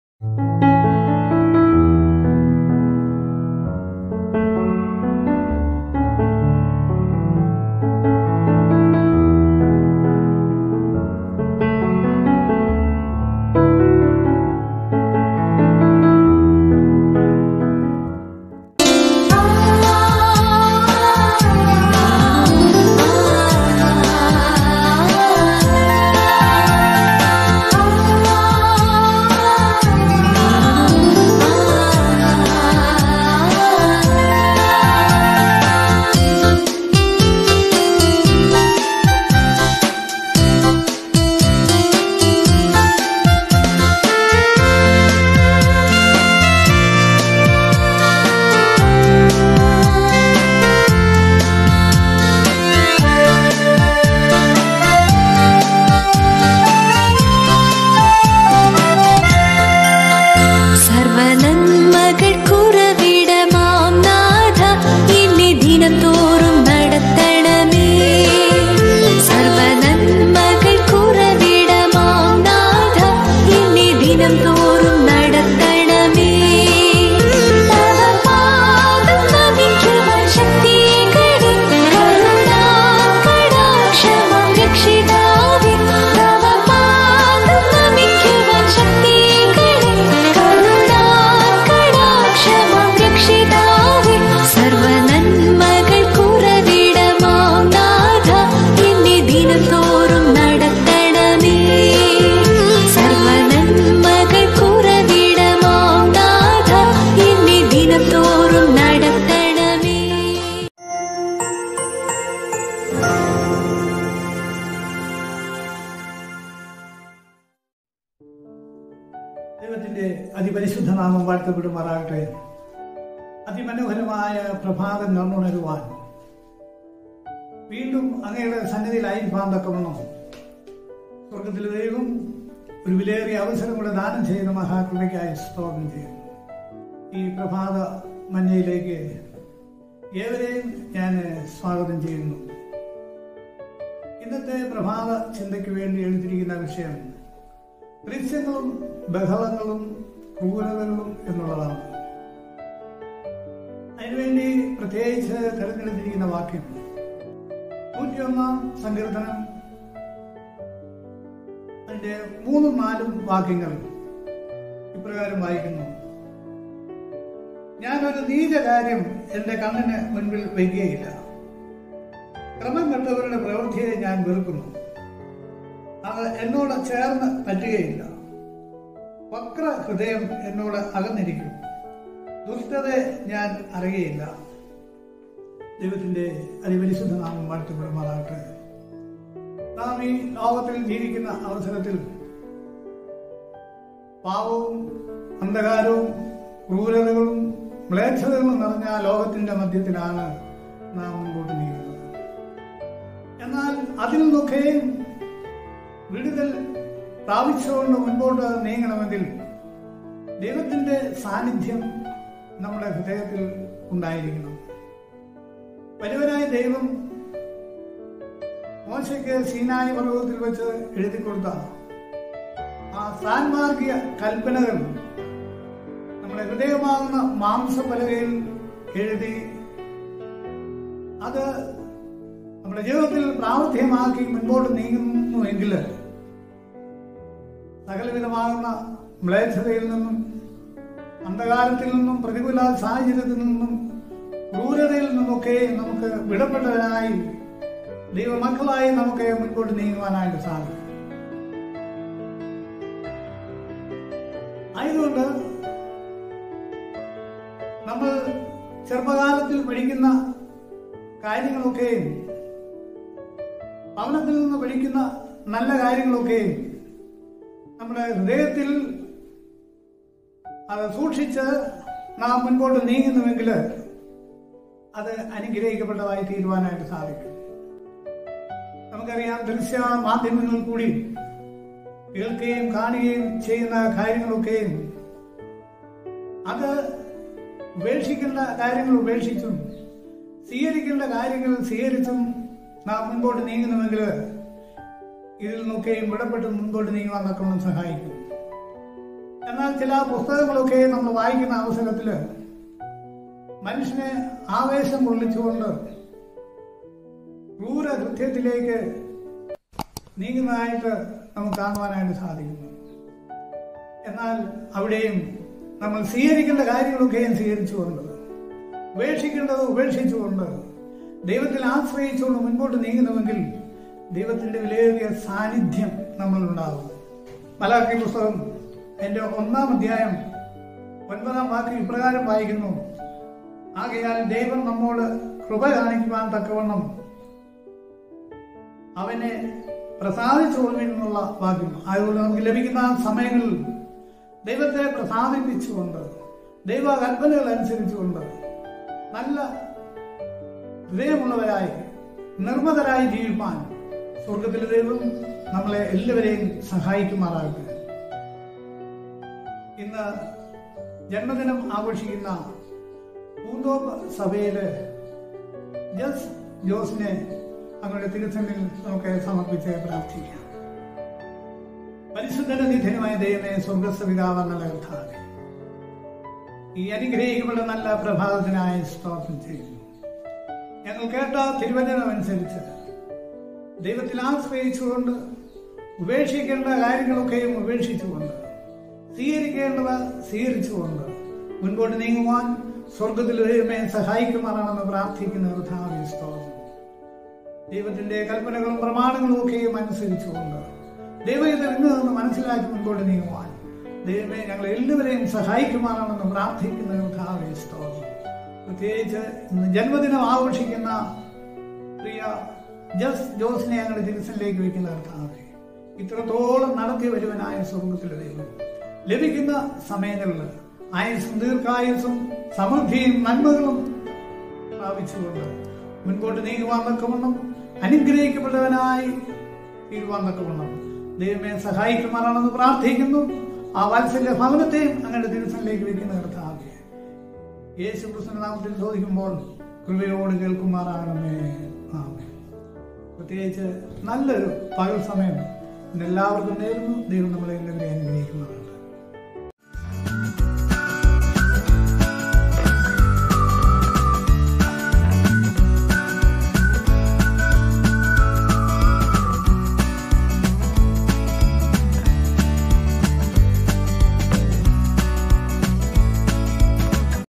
Morning Manna /Sights ,Sounds and Criminality/ Pr. V K John / 17 May 2021/Malayalam Morning Devotion
Hello Brethren,
Greetings to you in the name of our Lord from Alappuzha-Pathanamthitta Section Headquarters.
As we are indoors on Sabbath Day due to the Corona virus.
May this be a medium to continue in our Sabbath Divine Hour and be spiritually nourished
Please watch and be blessed.

In case of any prayer requests, needs and other emergencies in this lockdown time.